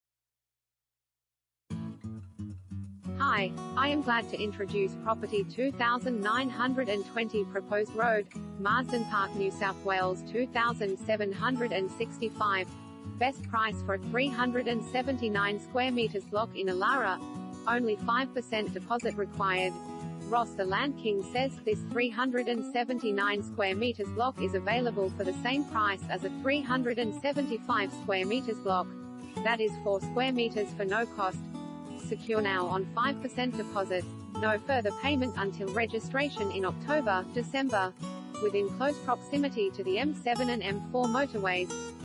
2920 Proposed Road, Marsden Park NSW 2765 | AuPropertyReport.Com
2920 Proposed Road, Marsden Park NSW 2765
BEST PRICE for a 379m2 block in Elara. Only 5% deposit required.